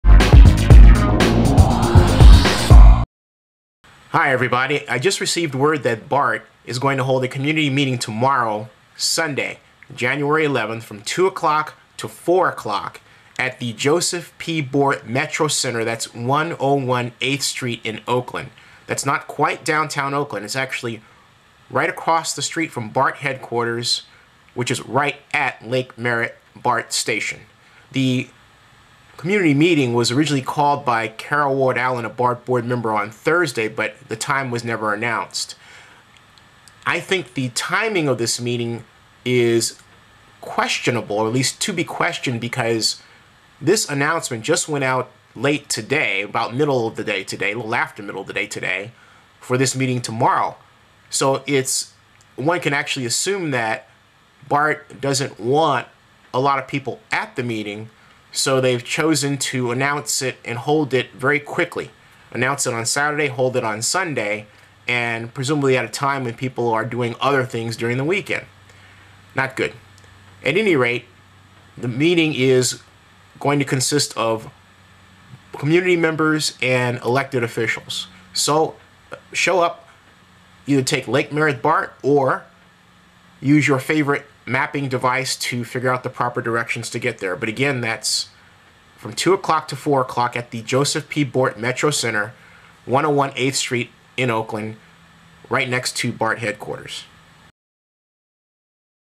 BART Meeting on Oakland Shooting Sunday Jan 11 2 PM to 4 PM
Zennie62 I was just informed today, Saturday evening January 10th, 2009, that BART is holding a "community meeting" on Sunday, January 11th, which is tommorrow, from 2 PM to 4 PM at 101 Eighth Street, the Joseph P. Bort MetroCenter in Oakland, off Lake Merritt BART - vlog produced by Zennie Abraham Zennie62 Blog - youtube video